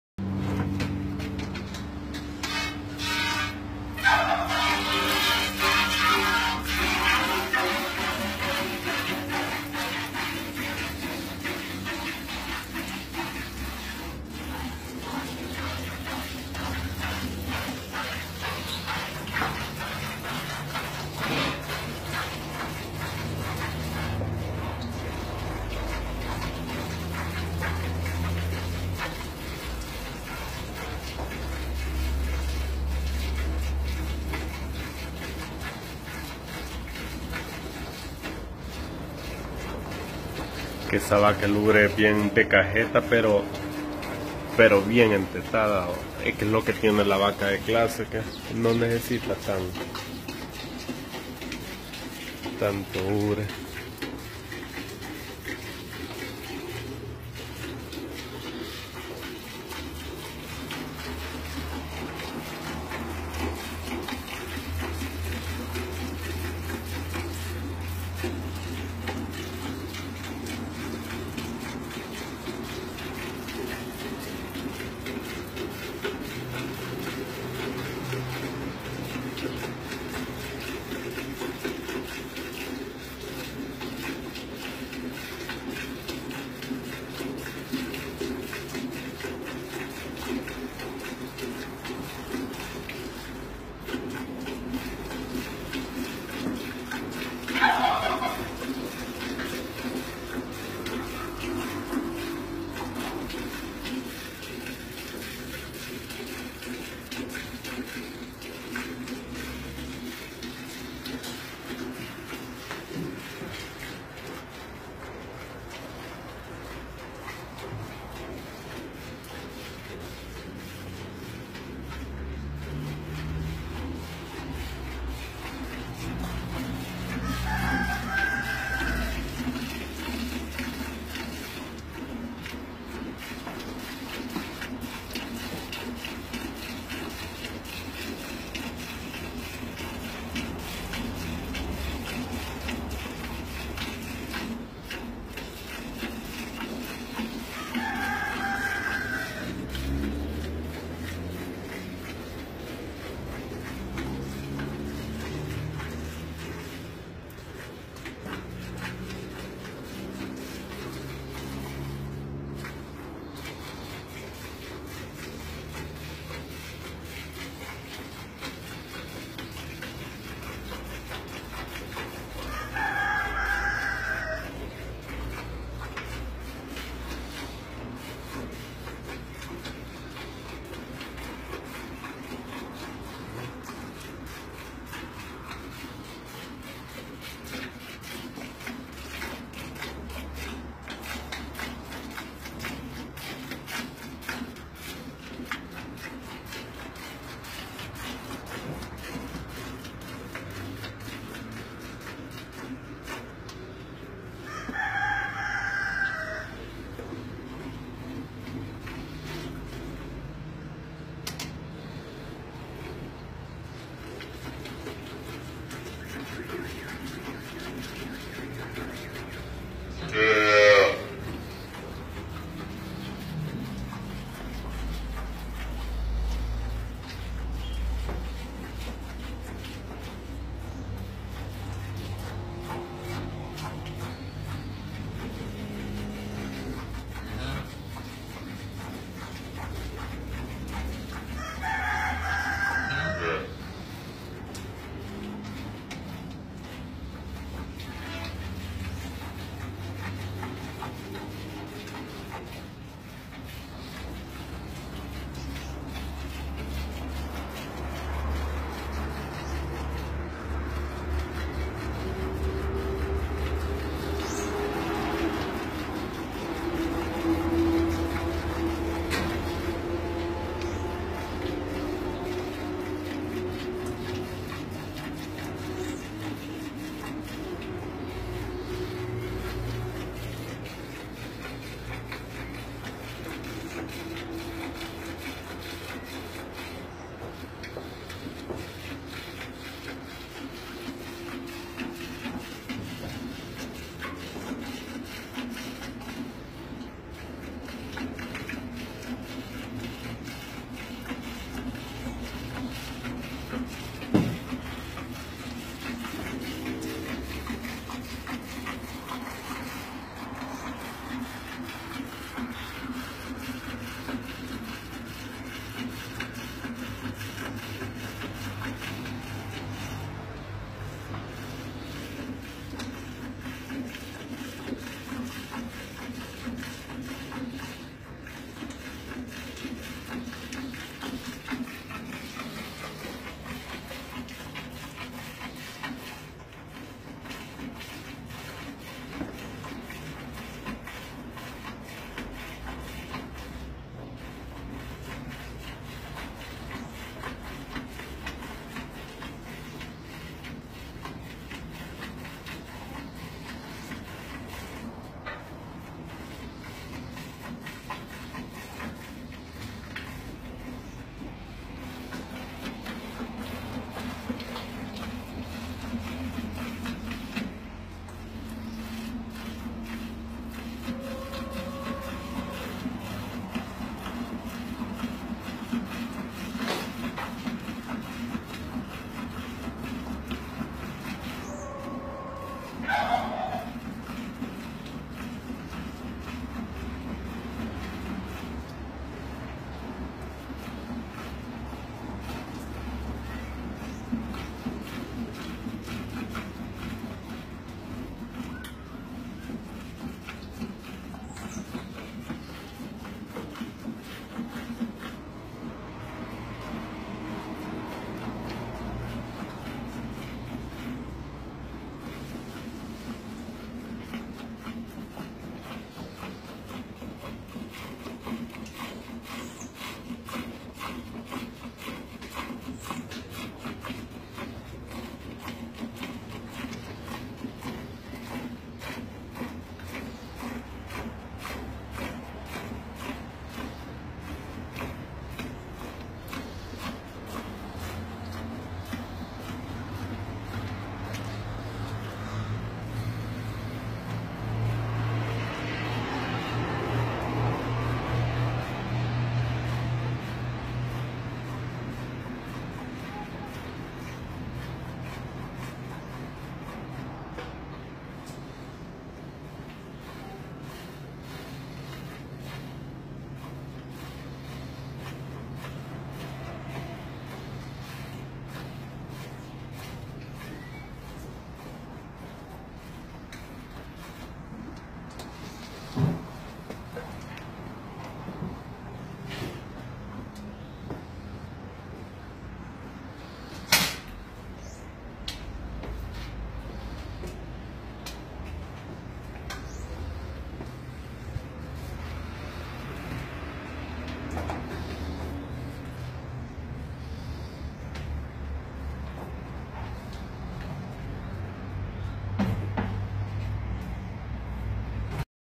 ORDEÑO VACA HOLSTEIN, 15 litros (18 botellas)
Video prestados por la placita ganadera